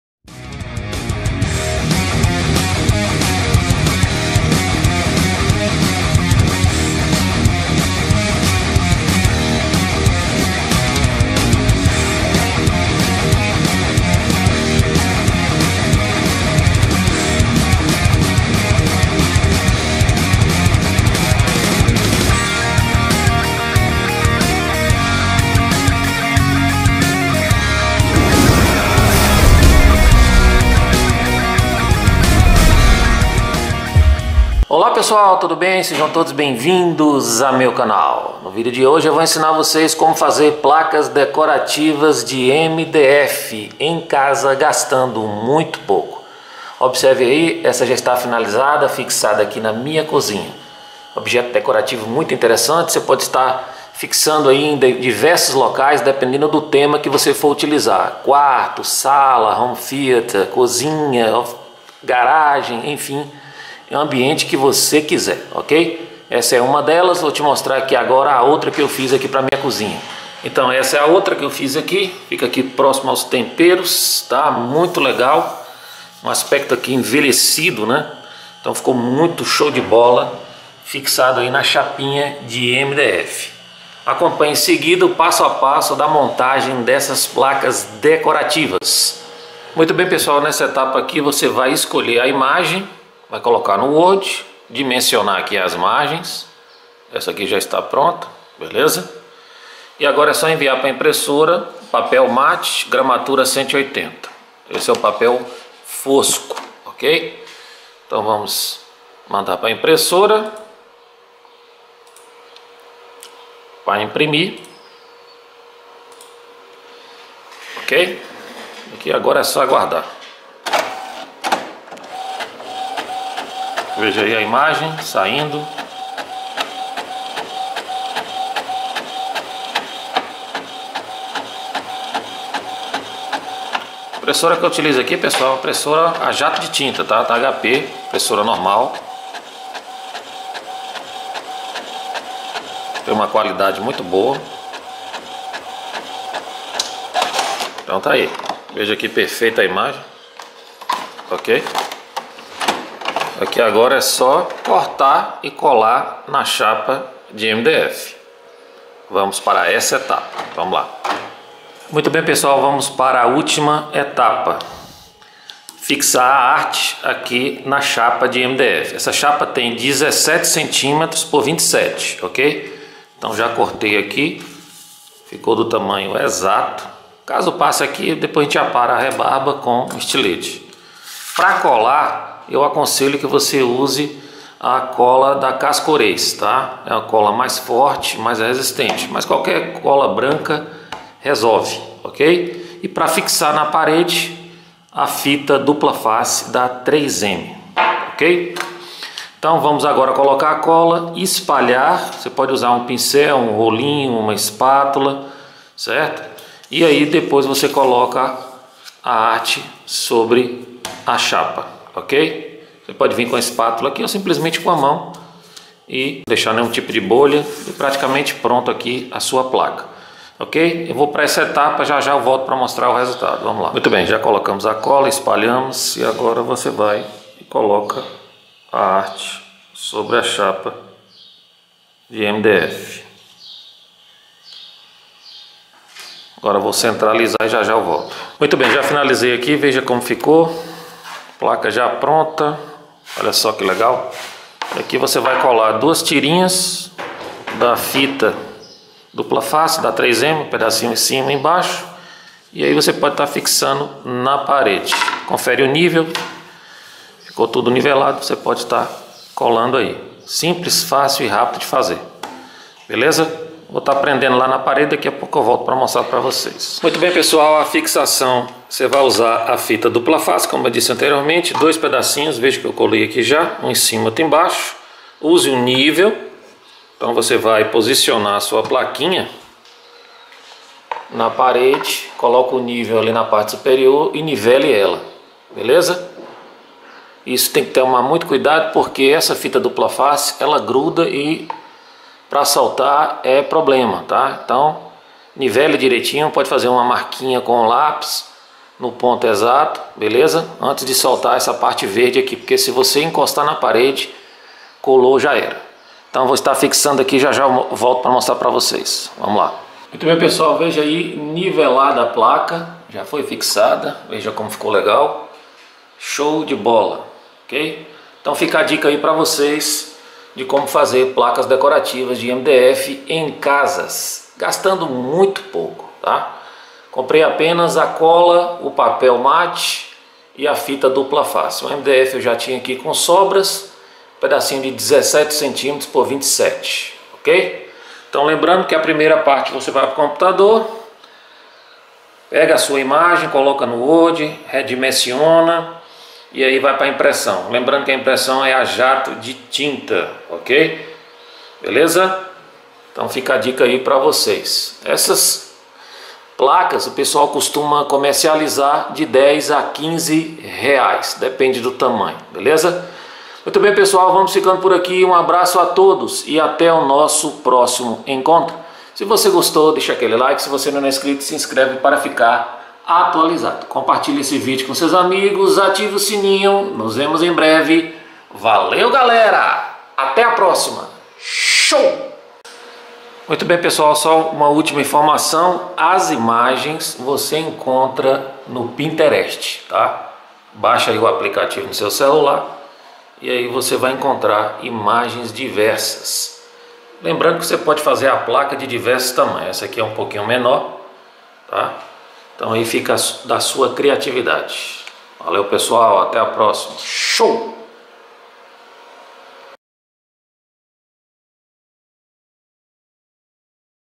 Como Fazer PLACAS DECORATIVAS de MDF - GANHE DINHEIRO !
Neste vídeo ensino passo a passo como fazer placas decorativas de mdf em casa gastando muito pouco e ainda vc pode vender obtendo um ótimo lucro.